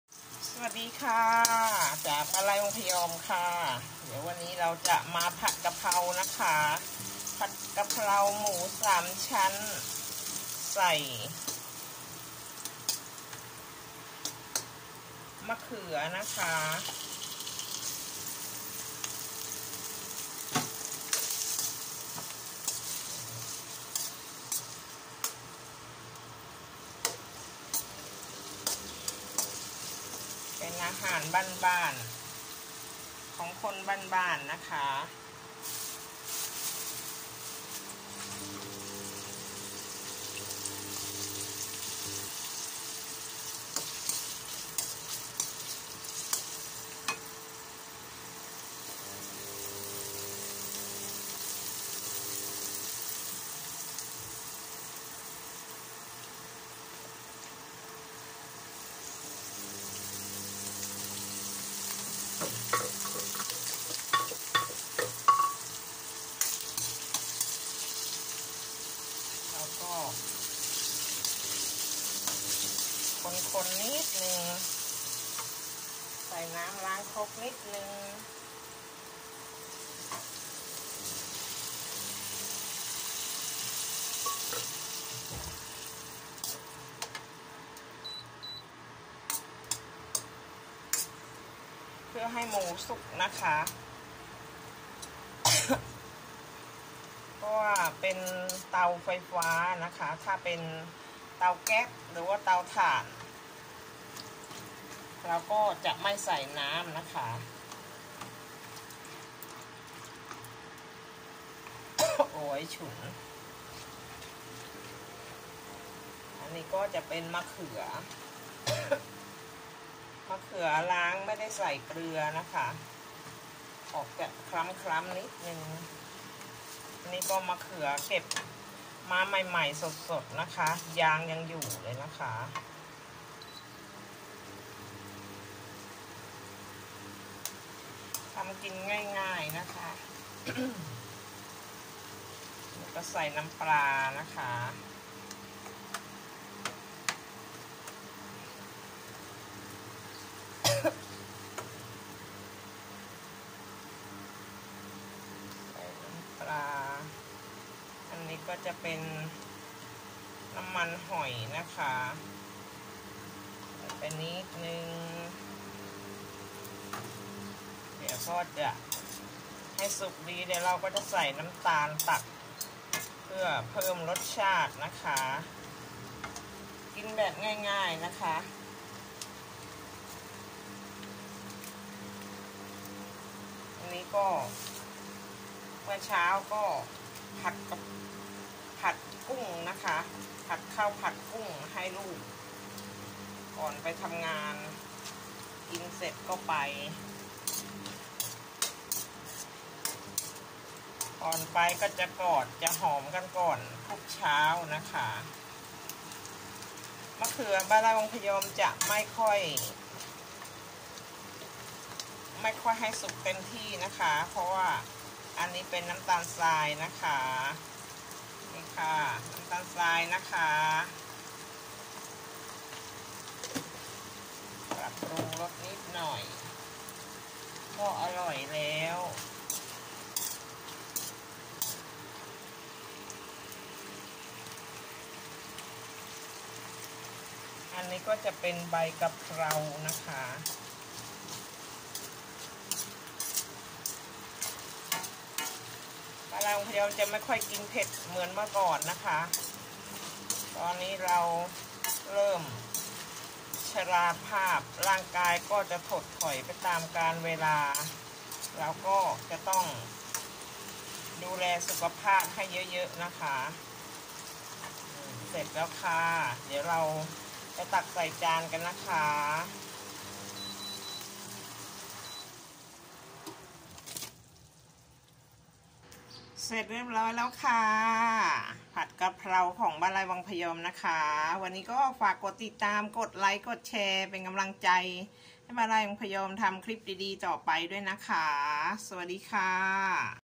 ผัด​กะเพรา​บ้านทุ่งบนยอดตึก!! ที่​💞บ้านไร่​วัง​พ​ยอม​💞
คลิป​นี้​เป็นความในใจ​ที่อัดอั้นตันใจ​ กลางเมือง​ใหญ่​ ใครๆมัก  จะค่อนแคะ  ว่า  สิ้นคิด  ติดข้างทาง  อ้างไม่รู้​ จะกินอะไร​ แต่จริงๆแล้ว  อร่อยถูกปาก  แต่ชอบถากถางคนทำกิน😂😂😂